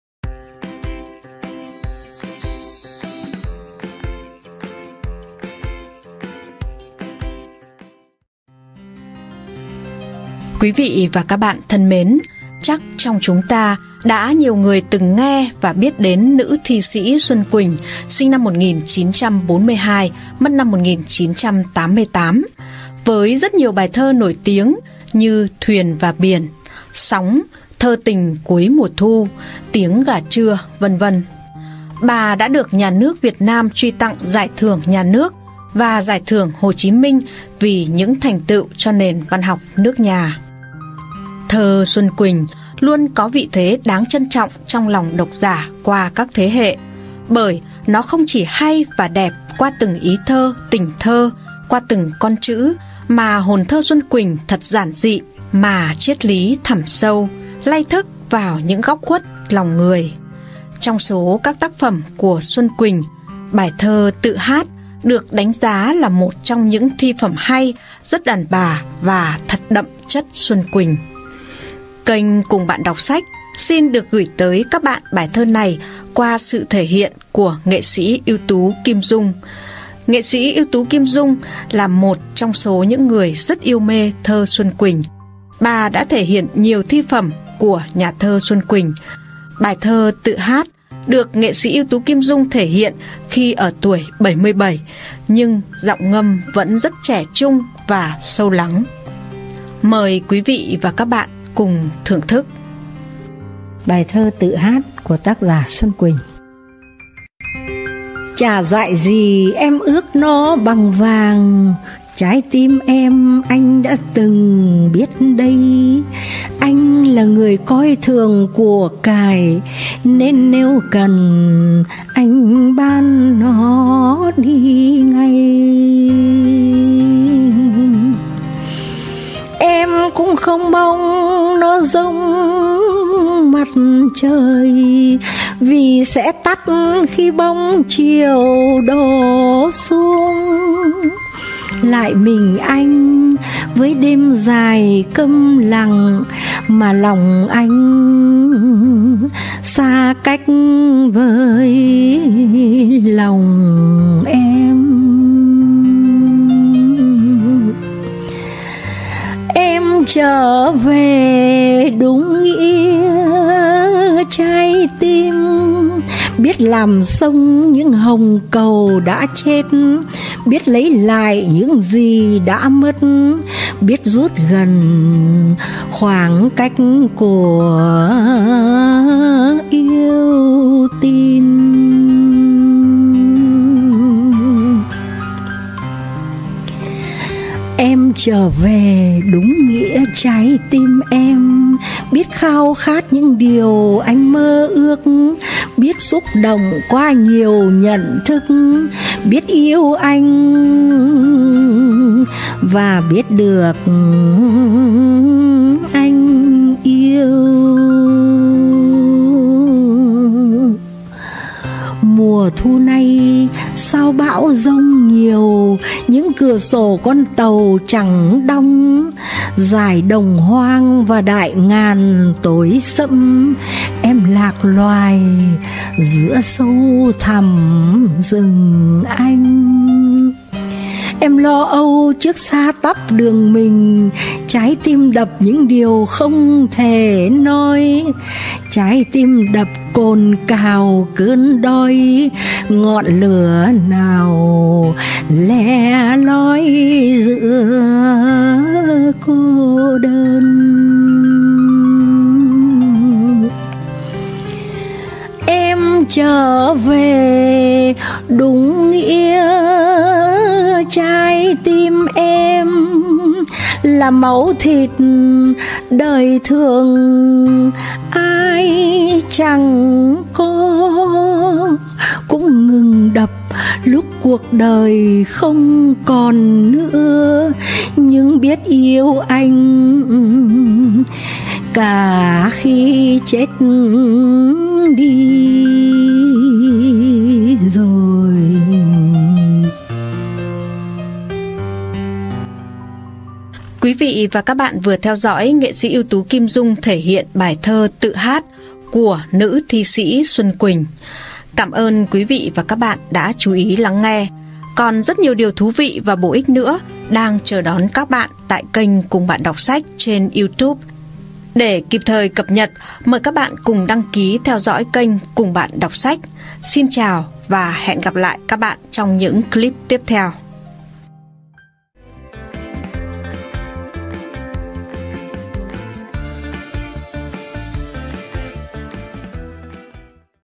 ĐỌC SÁCH CÙNG BẠN | Bài thơ - Tự hát - Xuân Quỳnh
Bài thơ: Tự hát
Tác giả: Xuân Quỳnh
Nghệ sĩ ưu tú Kim Dung: Diễn đọc
Thực hiện: Cùng Bạn Đọc Sách